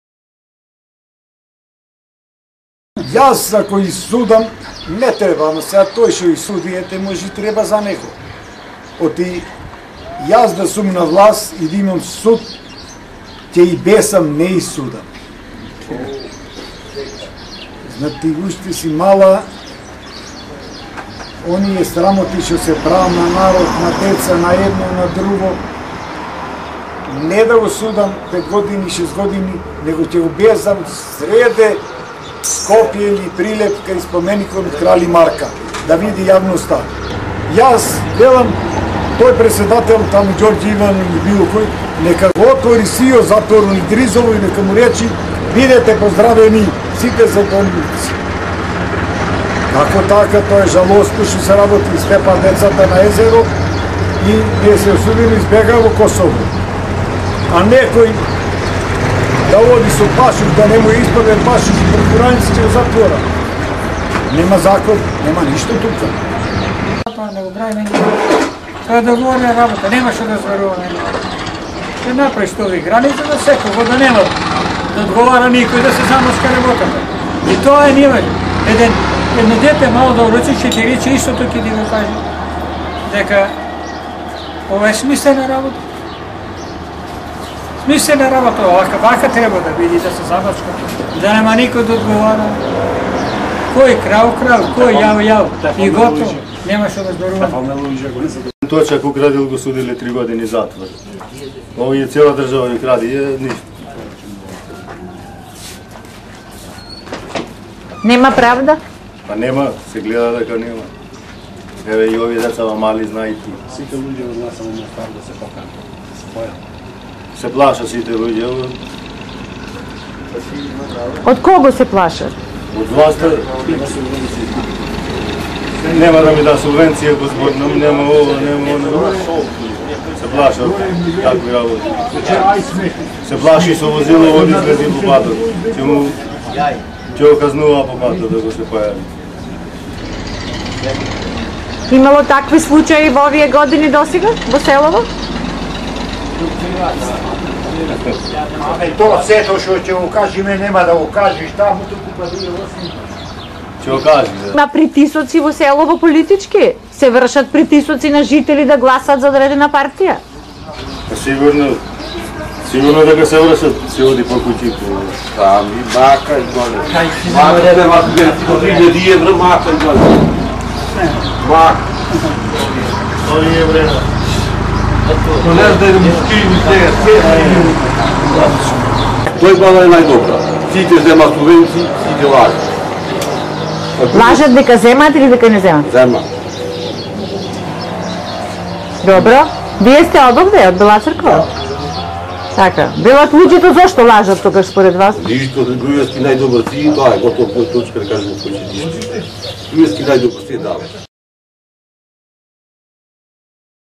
Политичка пролет во Бела Црква
Овде, во кривогаштанското село Бела Црква, сите жители се занимаваат со земјоделство, зашто едвај да има некој вработен. И оваа пролет, од утро до мрак тие се по нивите, освен во празник и недела, ден како овој, кога ги затекнавме пред продавницата среде село, како муабетат за животот, за политичките случувања, за секој следен чекор на политичарите, партиите...
И не ги кријат ставовите. Откако опозиционата СДСМ како што велат, започна со објавување на „бомбите“, а особено по одлуката за аболиција, што ја донесе претседателот Ѓорге Иванов, немаат дилеми околу изборниот процес. Сите со кои разговаравме на оваа тема се едногласни дека ќе гласаат, но само на кредибилни избори, тогаш кога ќе биде исполнет Договорот од Пржино.